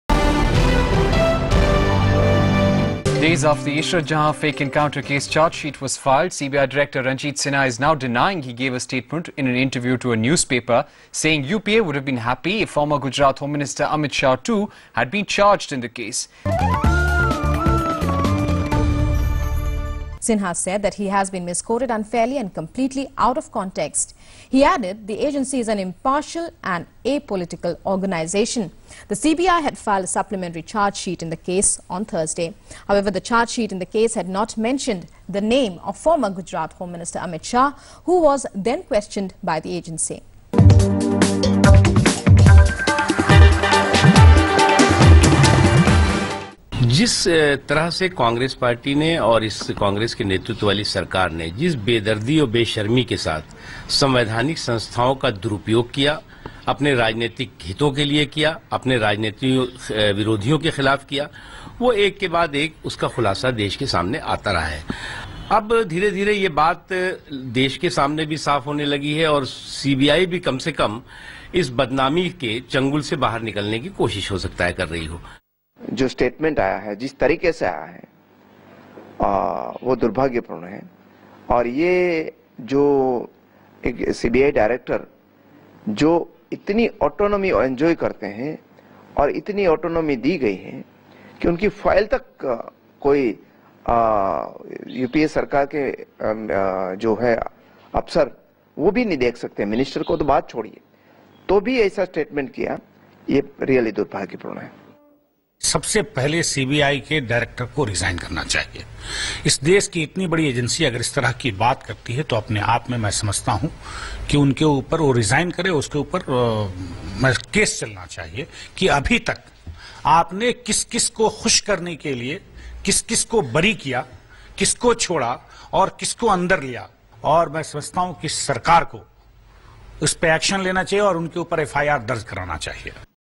Ishrat Jahan case: CBI says UPA never pressured to prosecute Shah
CBI director Ranjit Sinha denied the news that he was under pressure of the UPA government to prosecute BJP leader Amit Shah in the Ishrat Jahan fake encounter case. He also said that he never gave the statement that the UPA would have been happy if Amit Shah too had been charged in the case. Sinha added that agency didn't charge Shah because there was no prosecutable evidence.